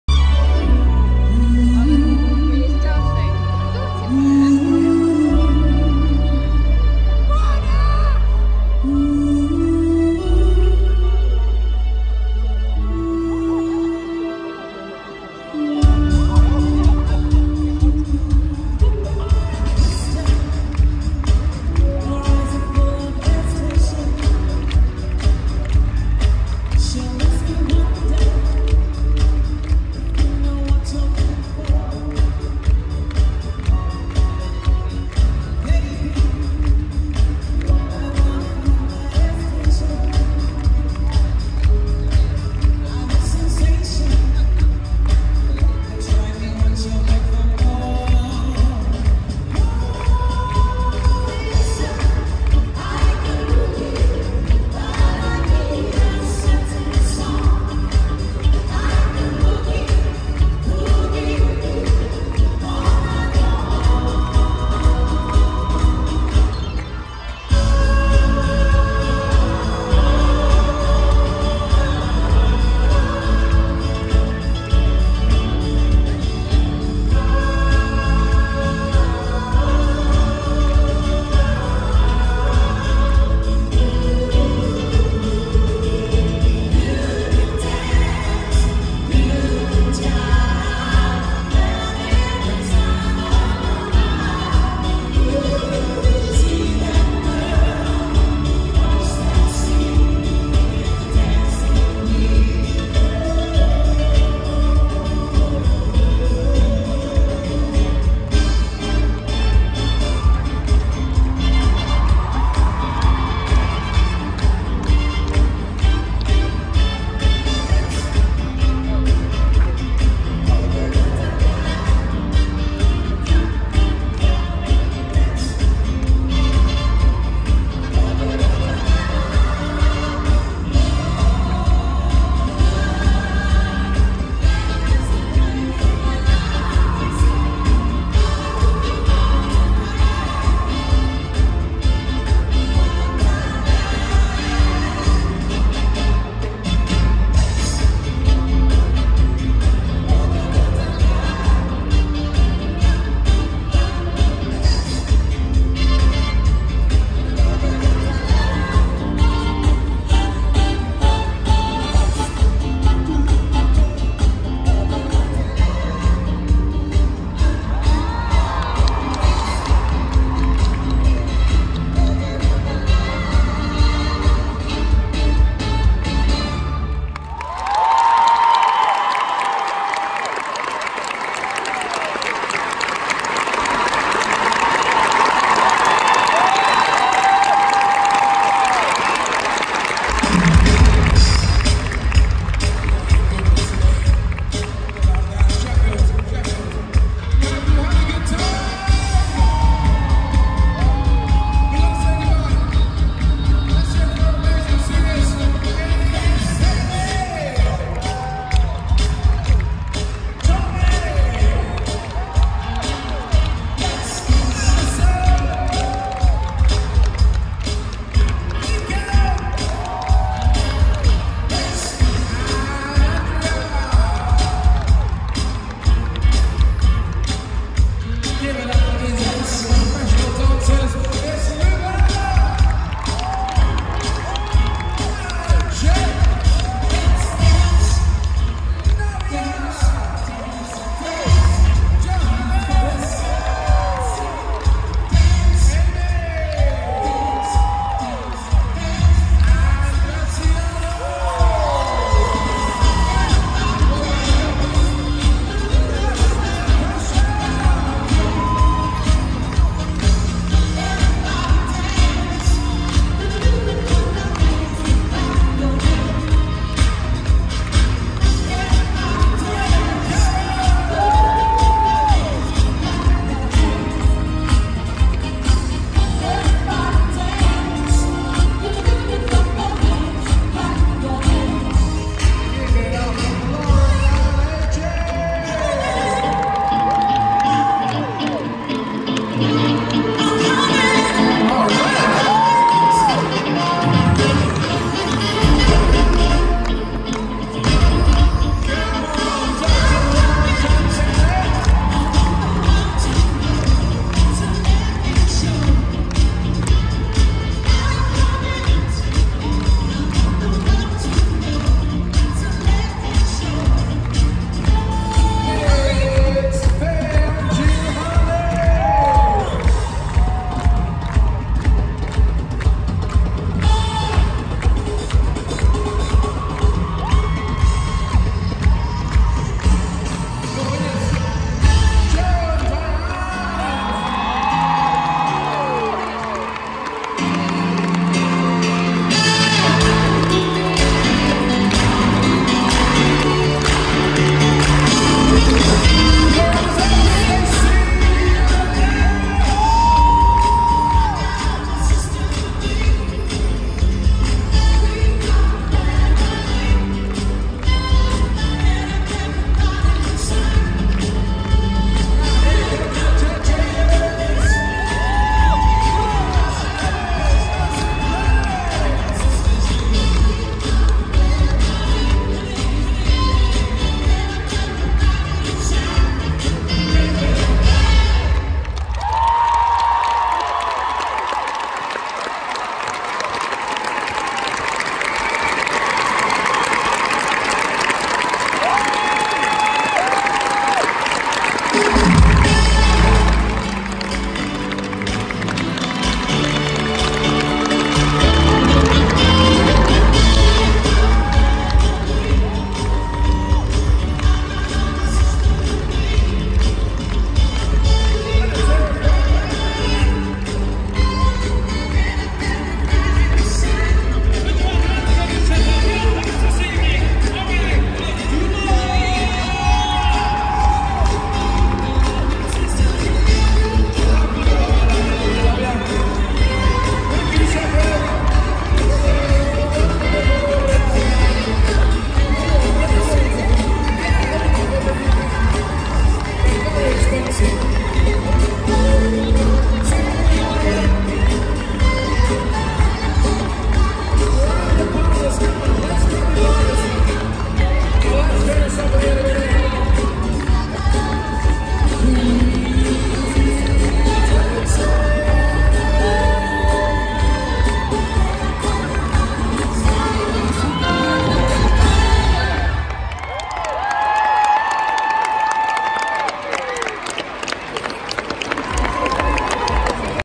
Strictly Come Dancing Tour - Finale including Judges Dancing - Sheffield 30th January 2019
Strictly Come Dancing Tour Finale including the Judges Dancing - 30th January 2019, Fly DSA Arena Sheffield

Block 103 Row F